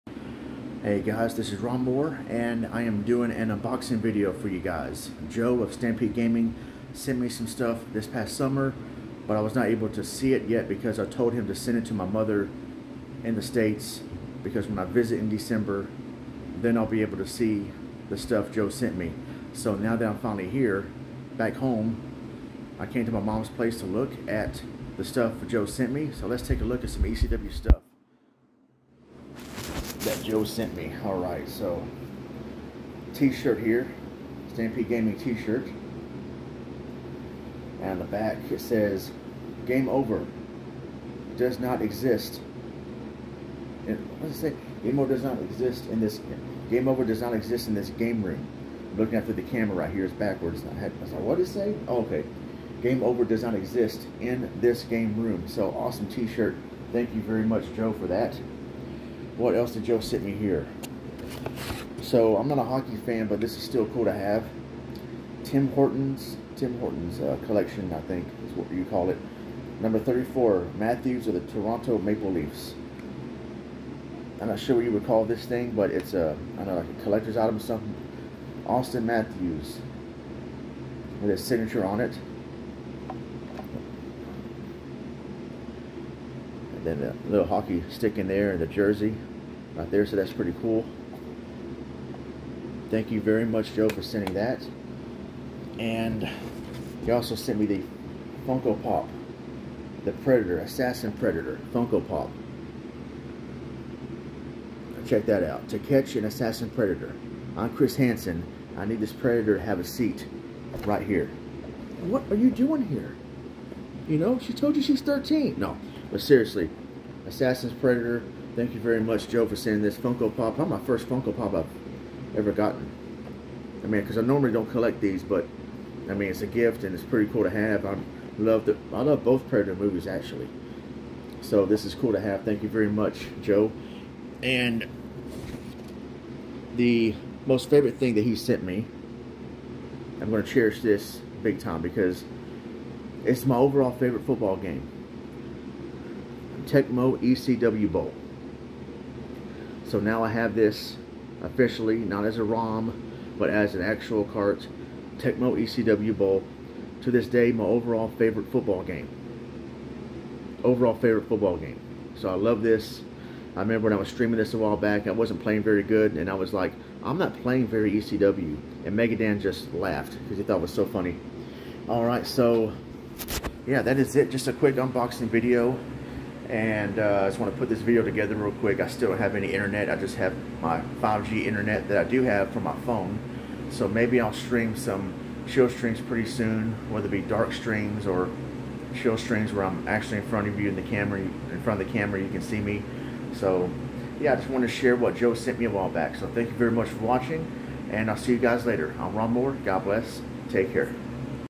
Gifts from Stampede Gaming!
Stampede Gaming sent me some gifts a while back after I won a raffle in his stream.  I am finally able to unbox them!